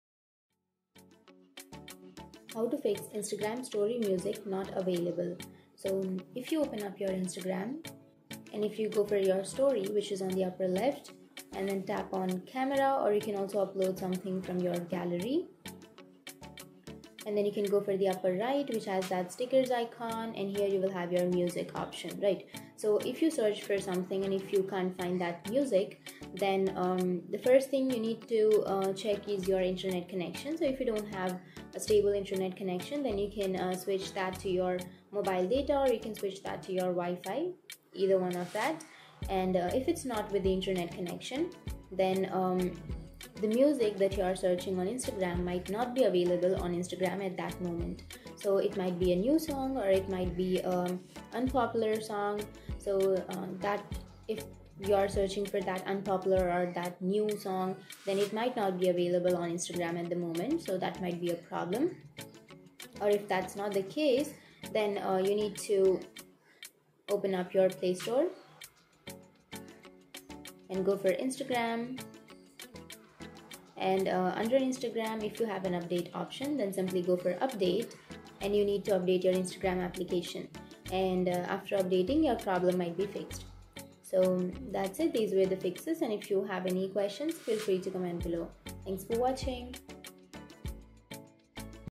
How To Fix Instagram Story Music Not Available?
Welcome to QuickFixTutorials, your reliable source for resolving tech glitches. In this guide, we'll tackle a common Instagram woe: "Music Not Available" on your story. If you've been struggling to add tunes to your Instagram stories, we've got your back. Follow our step-by-step instructions to troubleshoot and fix the "Music Not Available" problem and get your stories grooving again.

Timestamps:
00:10 - Introduction: The Instagram Story Music Challenge
00:35 - How to Diagnose "Music Not Available" Issue
01:05 - Step-by-Step Fixes for Instagram Story Music Problem